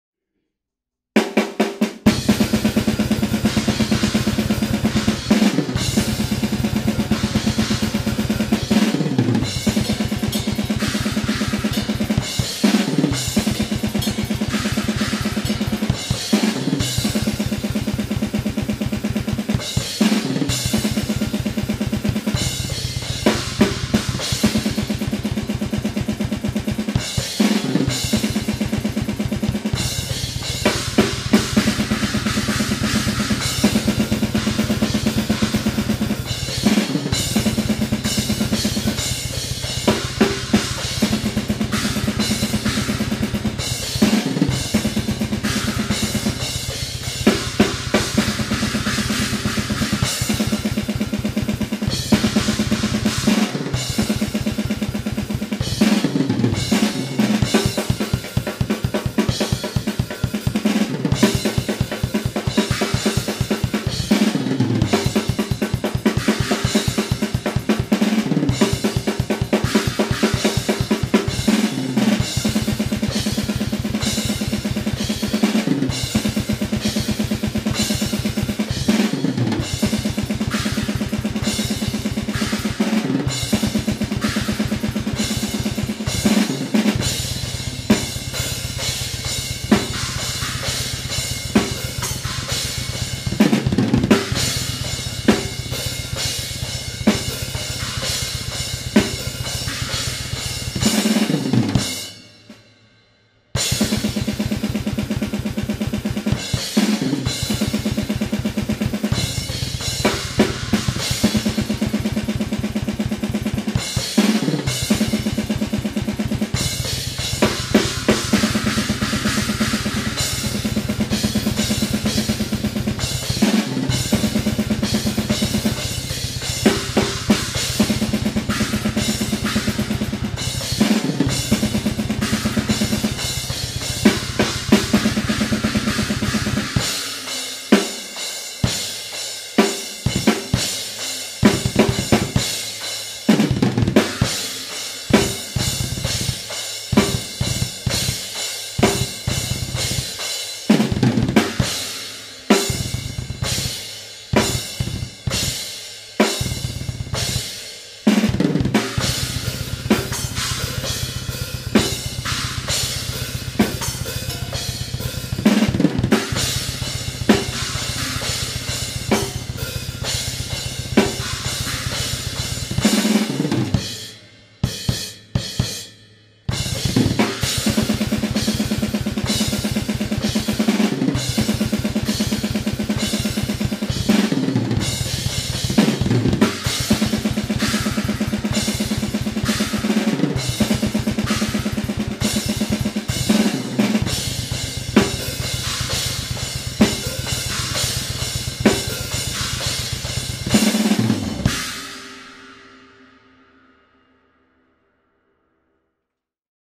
260 bpm one foot blast/hyper blast, no triggers - James Payne
Just a quick video while I was rehearsing my band Hiss From The Moat's song from 2009. The song is 260 bpm and the recording is 100% natural sound, no triggers, no microphones, no editing.